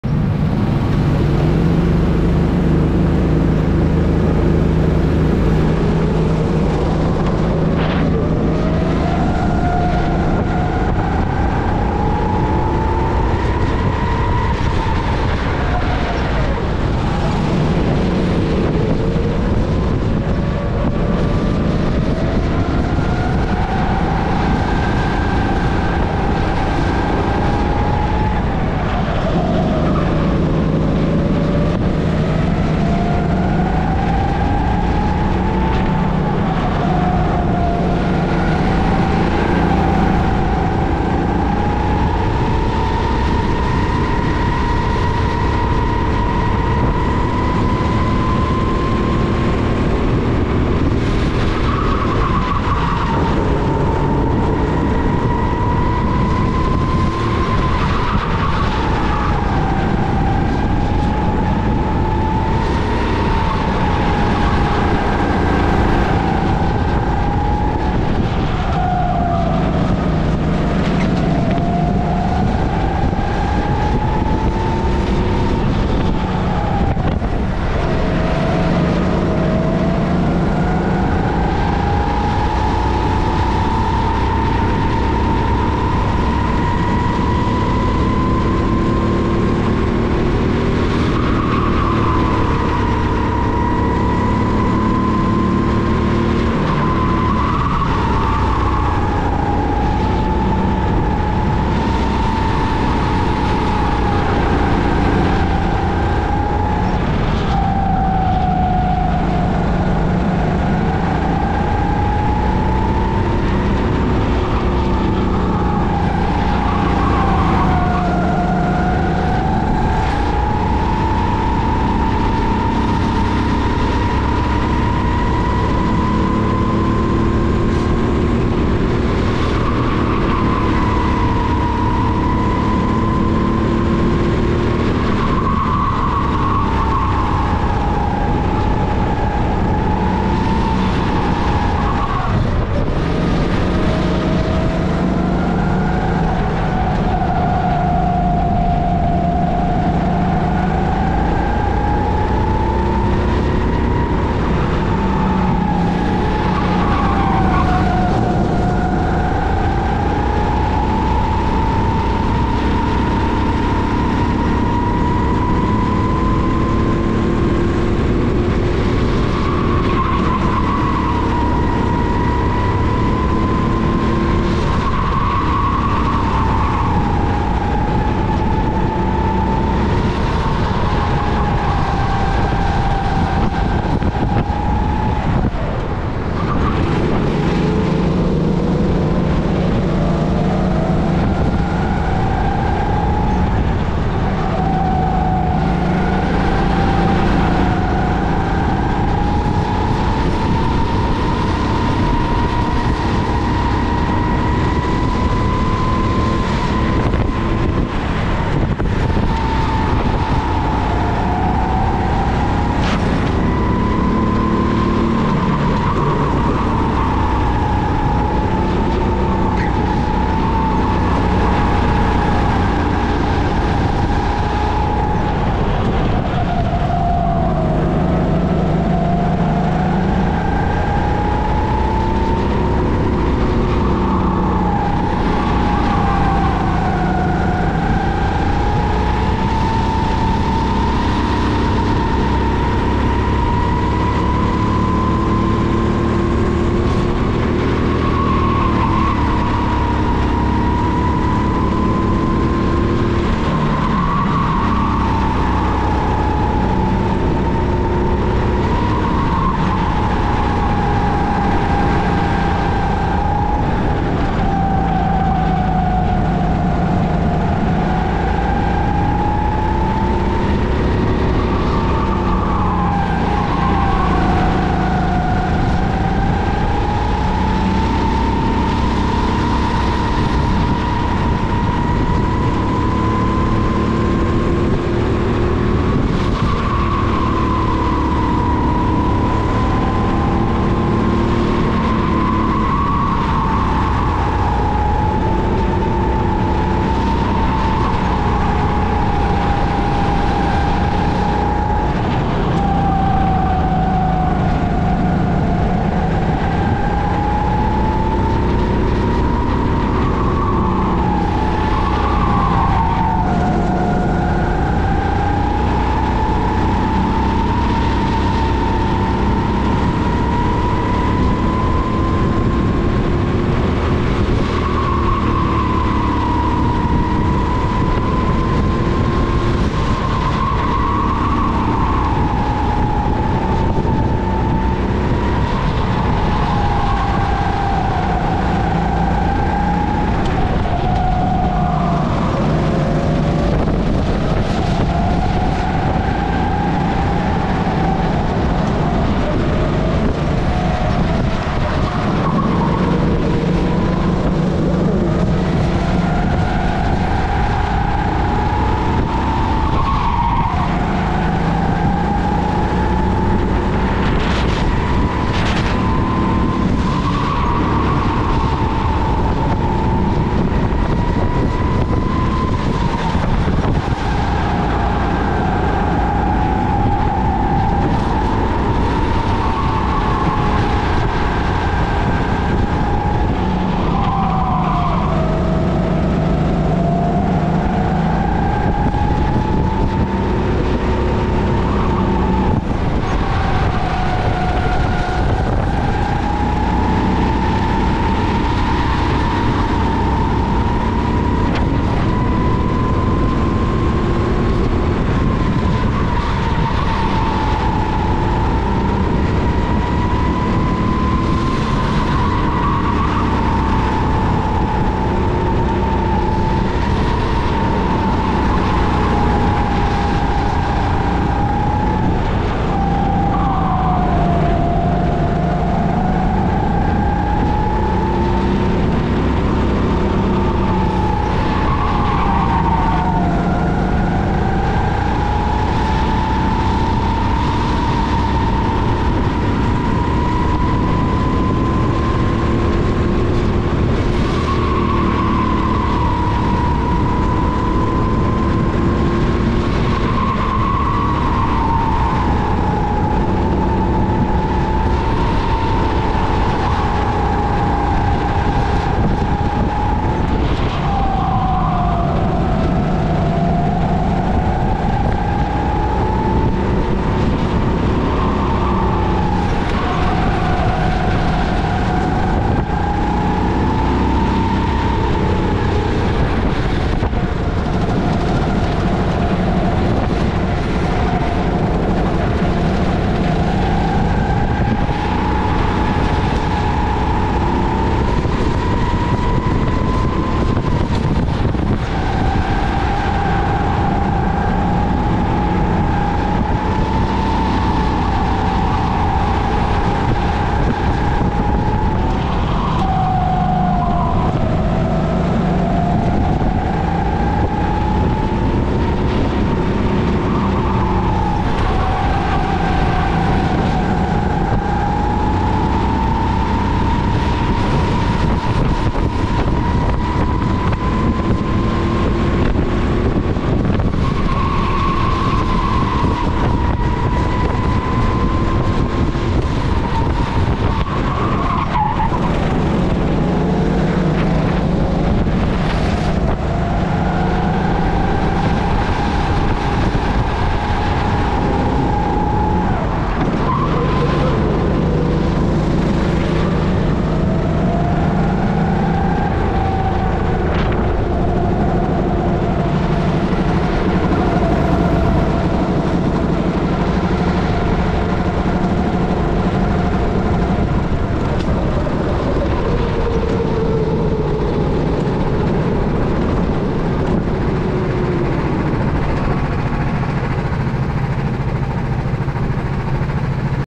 Go-Karting - Kartcircuit Adventure Eefde - Heat 20 - 06-AUG-2022
Track: Kartcircuit Adventure Eefde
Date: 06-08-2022
Heat: 20
Camera: GoPro Hero 10
Camera position: helmet

Lap times:

01. 00:39.59
02. 00:38.50
03. 00:39.69
04. 00:39.75
05. 00:39.05
06. 00:38.35
07. 00:38.17 *
08. 00:42.08
09. 00:39.12
10. 00:38.62
11. 00:39.41
12. 00:40.55

Average lap time: 00:39.41